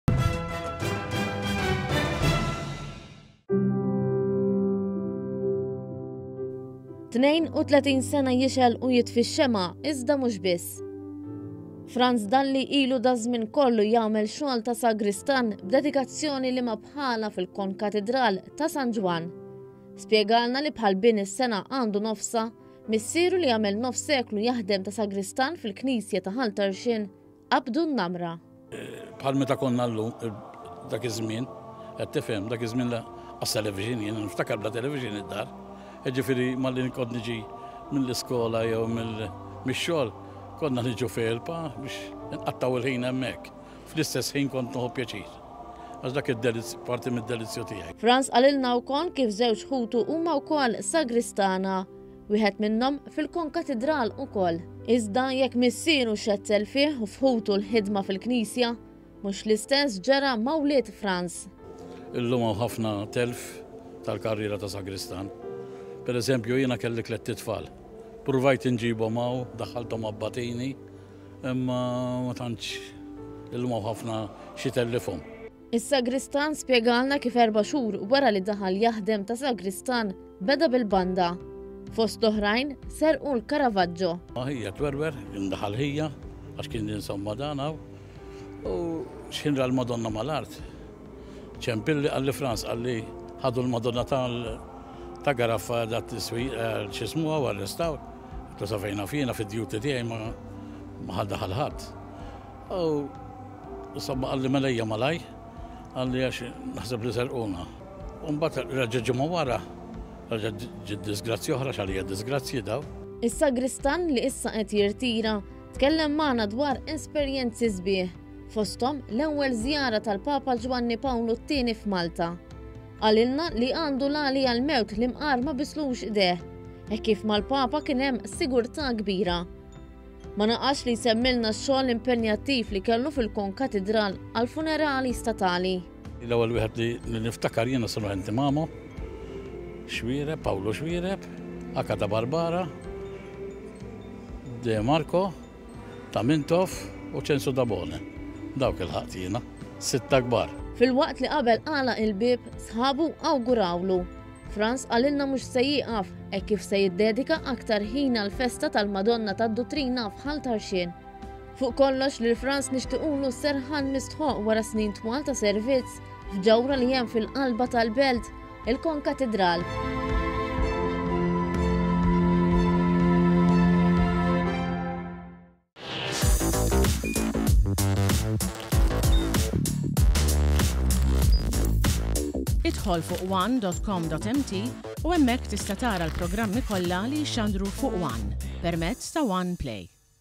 JIRTIRA S-SAGRISTAN TAL-KON-KATIDRAL LI BEDA BIL-BANDA
ONE.COM.MT - Beda bil-banda iżda qatt ma ħares lura.
Illum ġiet fi tmiemha karriera ta’ tnejn u tletin sena ta’ sagristan fil-Kon Katidral ta’ San Ġwann.
Din hija l-istorja ta’ Frans Dalli li b’dedikazzjoni kbira ħa ħsieb ġawhra li mhux biss tgħaxxaq lill-Maltin iżda anki lil bosta turisti li jżuruwha.
KITBA: LEANNE ABELA GRECH
FILMAT: CHRIST BORG
Tnejn u tletin sena jixgħel u jitfi x-xemgħa iżda mhux biss.
Frans Dalli ilu daż-żmien kollu jagħmel xogħlu ta’ sagristan b’dedikazzjoni liema bħala fil-Kon-Katidral ta’ San Ġwann.

Spjegalna li bħal ma bin is-sengħa għandu nofsha, missieru li għamel nofs seklu jaħdem ta’ sagristan fil-Knisja ta’ Ħal Tarxien, qabbdu n-namra.
 FRANS DALLI
            SAGRISTAN IRTIRAT
 “Mhux bħalma meta konna qabel, dak iż-żmien lanqas television, niftakar bla television id-dar, jiġifieri malli kont niġi mill-iskola jew mix-xogħol konna niġu fejn il-pa biex inqattgħu l-ħin hemmhekk, fl-istess ħin kont nieħu pjaċir għax dak parti mid-delizju tiegħi.”

Frans qalilna wkoll kif żewġ ħutu huma wkoll sagristana, wieħed minnhom fil-Kon-Katridal ukoll.
Iżda jekk missieru xettel fih u f’ħutu l-ħidma fil-Knisja, mhux l-istess ġara ma’ wlied Frans.
FRANS DALLI
           SAGRISTAN IRTIRAT
 “Illum hawn ħafna telf tal-karriera ta’ sagristan pereżemju jien kelli tlett itfal ppruvajt inġibhom hawn, daħħalthom abbatini imma ma tantx, illum hawn ħafna xi jtellifhom.”

Is-sagristan spjegalna kif erba’ xhur wara li daħal jaħdem ta’ sagristan, beda bil-banda.

Fost l-oħrajn serqu l-Caravaggio.

SFRANS DALLII
SAGRISTAN IRTIRAT
“Ħija twerwer kien daħal ħija u x’ħin ra l-Madonna mal-art ċempilli qalli Frans qalli ħadu lill-Madonna ta’ Caraffa għar-restawr. Għedtlu sa fejn naf jiena fid-duty tiegħi ma daħal ħadd. U qalli ejja malajr qalli għax naħseb li serquna. WHITE FLASH. U mbgħad ġimgħa wara reġgħet ġiet disgrazzja oħra, għax għalija disgrazzji dawn”

Is-sagristan li issa qed jirtira, tkellem magħna dwar esperjenzi sbieħ, fosthom l-ewwel żjara tal-Papa Ġwanni Pawlu t-Tieni f’Malta.

Qalilna li għandu għali għall-mewt li mqar ma bieslux idejh, hekk kif mal-Papa kien hemm sigurtà kbira.

Ma naqasx li jsemmilna x-xogħol impenjattiv li kellu fil-Kon-Katidral għall-funerali statali.

FRANS DALLII
SAGRISTAN IRTIRAT
“L-ewwel wieħed li niftakar jien ta’ Sir Anthony Mamo, Xuereb Pawlu Xuereb, Agatha Barbara, De Marco, ta’ Mintoff u Tabone, dawk ilħaqt jiena, sitta kbar.”

Filwaqt li qabel għalaq il-bieb, sħabu awgurawlu, Frans qalilna li mhux se jieqaf, hekk kif se jiddedika aktar ħin għall-festa tal-Madonna tad-Duttrina f’Ħal Tarxien.

Fuq kollox lil Frans nixtiequlu s-serħan mistħoqq wara snin twal ta’ servizz f’ġawhra li hemm fil-qalba tal-Belt, il-Kon-Katidral.